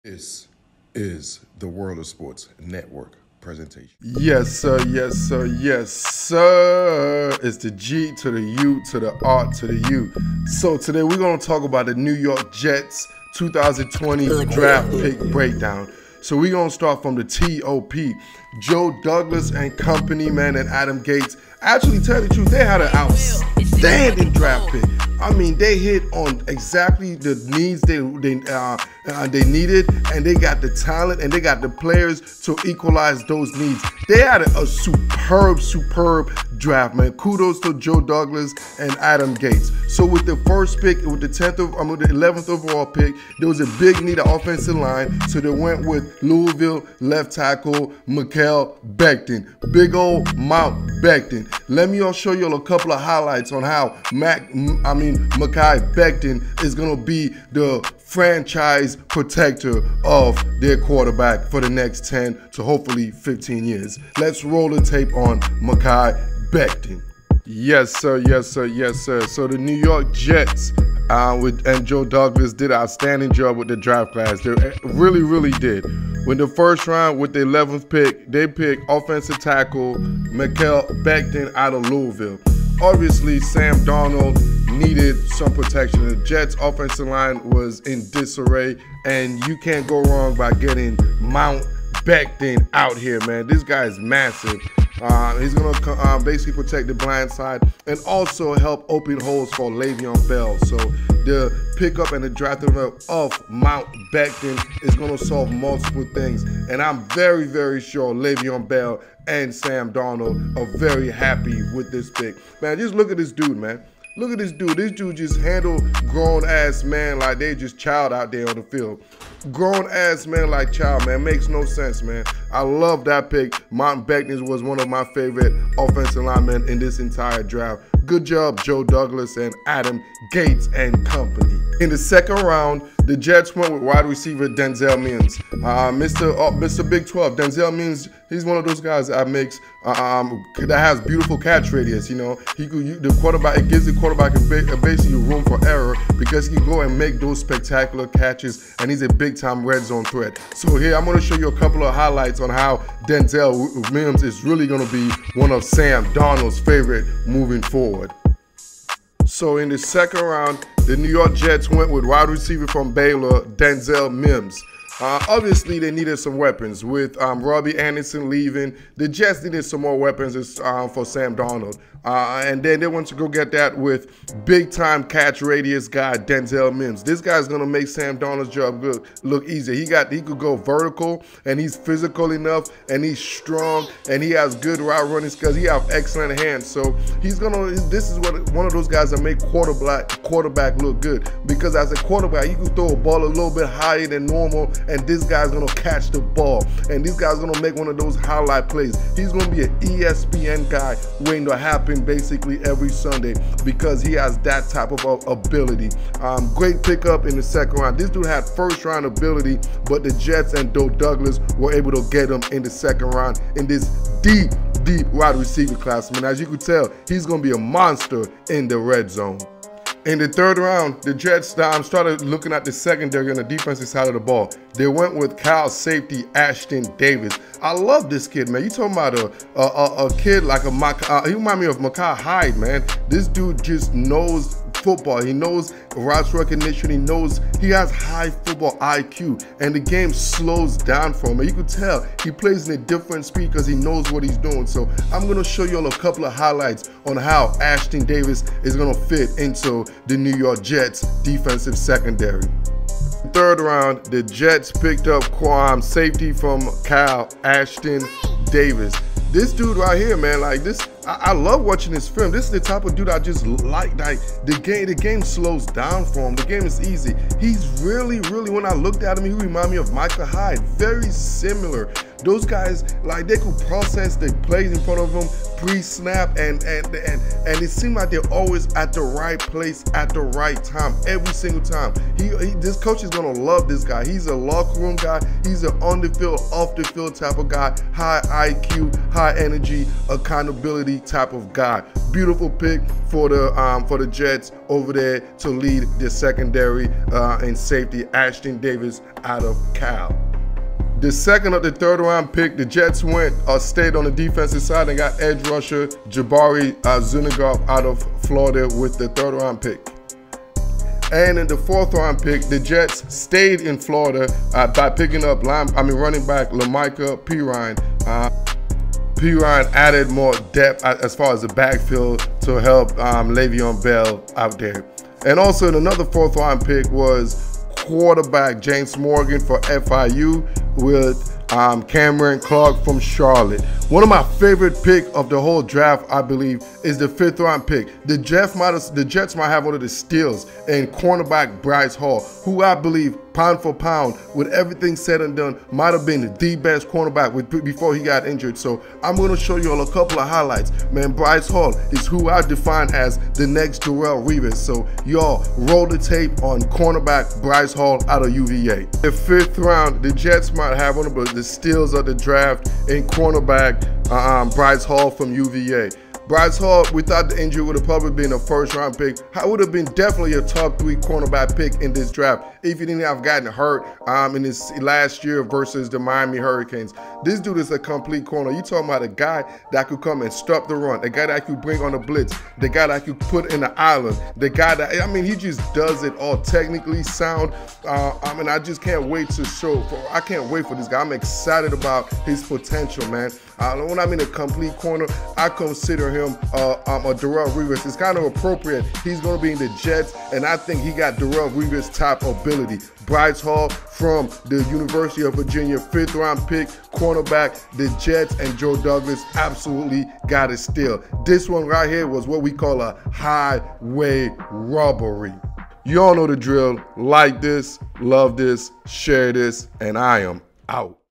NY Jets NFL DRAFT CLASS Breakdown
Round 1, pick 11: Mekhi Becton, OT, Louisville
Round 2, pick 59: Denzel Mims, WR, Baylor
Round 3, pick 68: Ashtyn Davis, S, California
Round 3, pick 79: Jabari Zuniga, DE, Florida
Round 4, pick 120: Lamical Perine, RB, Florida
Round 4, pick 125: James Morgan, QB, Florida International
Round 4, pick 129 (from Patriots via Ravens): Cameron Clark, OT, UNC Charlotte
Round 5, pick 158: Bryce Hall, CB, Virginia
Round 6, pick 191: Braden Mann, P, Texas A&M

If you like this Content you may also like these below